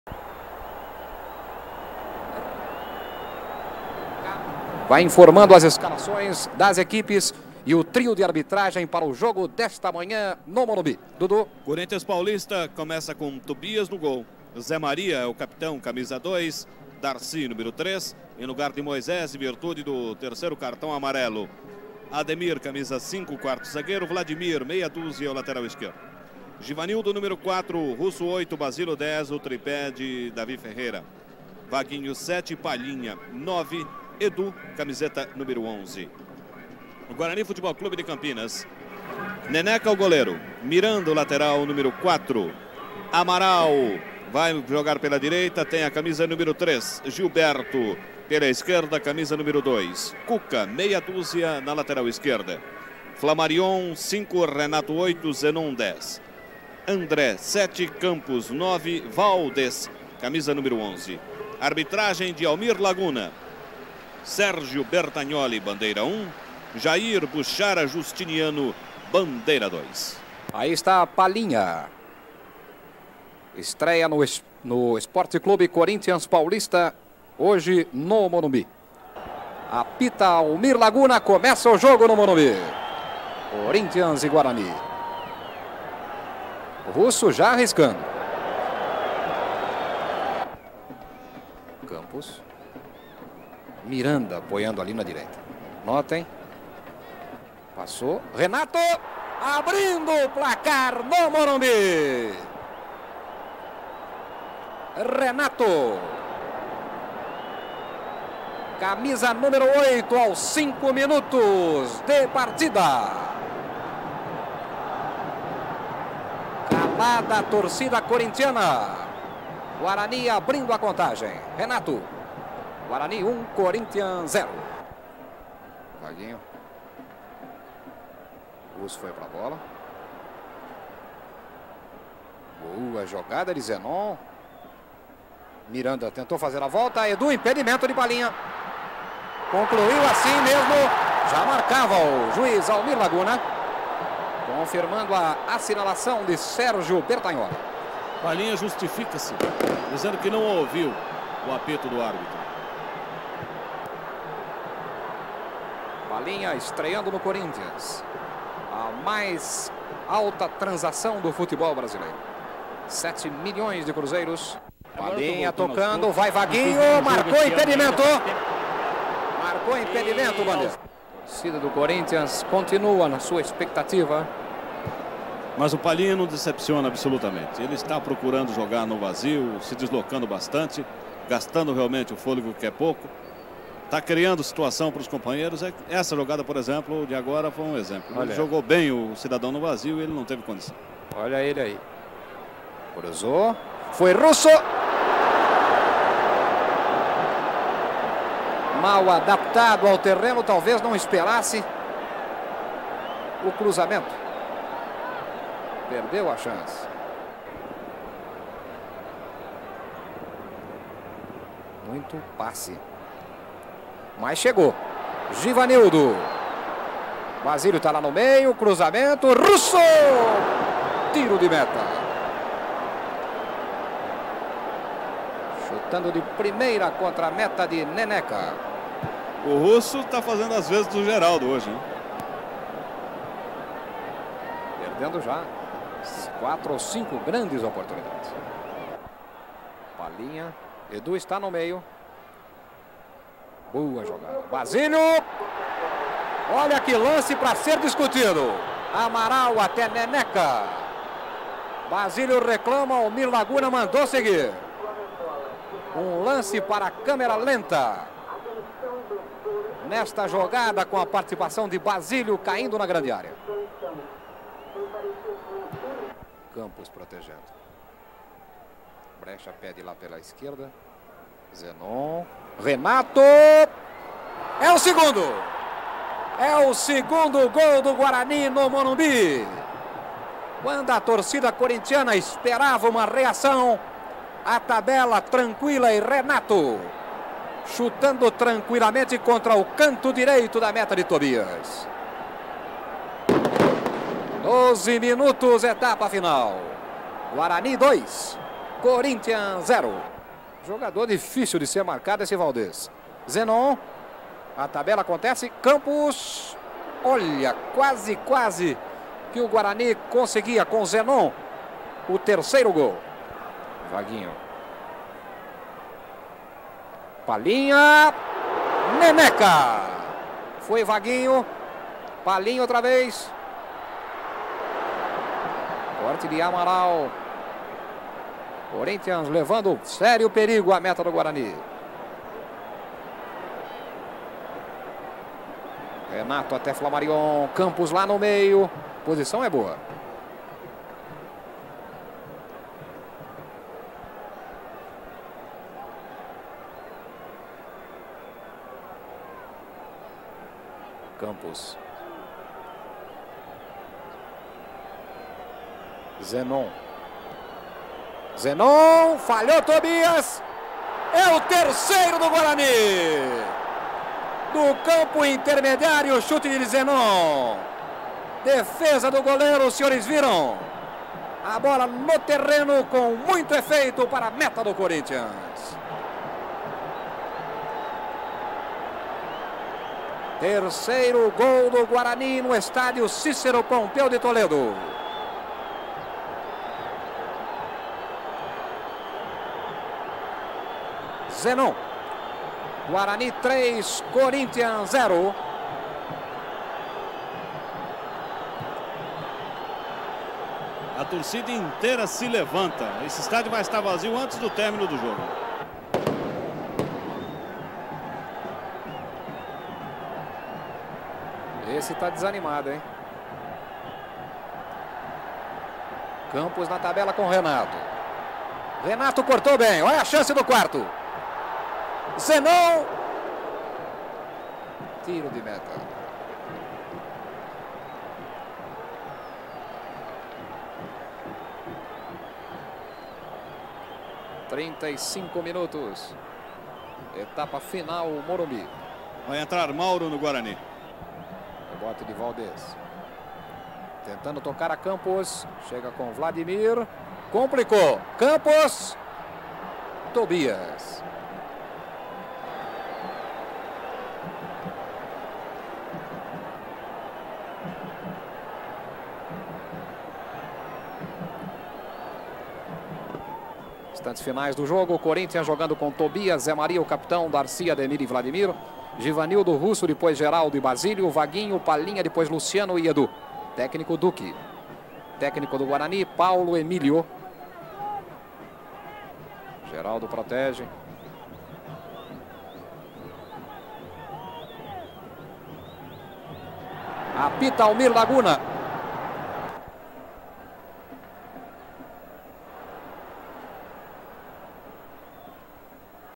Corinthians 0 x 3 Guarani - 27-03-1977 ( Campeonato Paulista )
Futebol Nostalgia trazendo os anos de ouro do futebol brasileiro e seus craques. Muitos gols e melhores momentos.

Corinthians 0 x 3 Guarani - Campeonato Paulista 1977